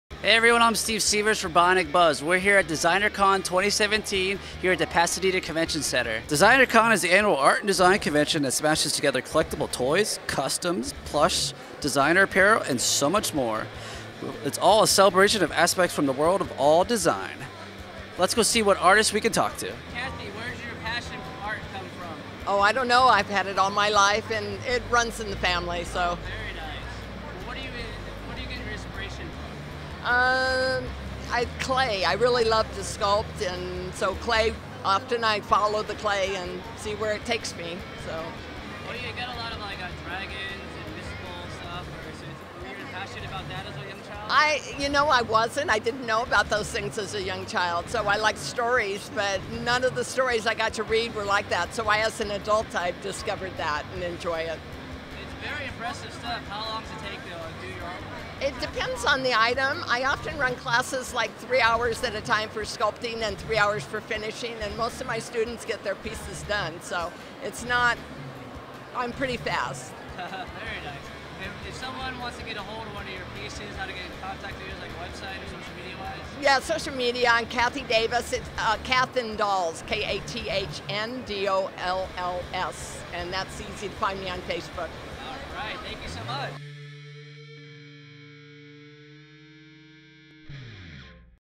DesignerCon 2017 Interview w/ Kathy Davis
Bionic Buzz® was covering DesignerCon 2017. An convention that celebrations everything in the world of art & design. There we got to interview artist & sculptor Kathy Davis.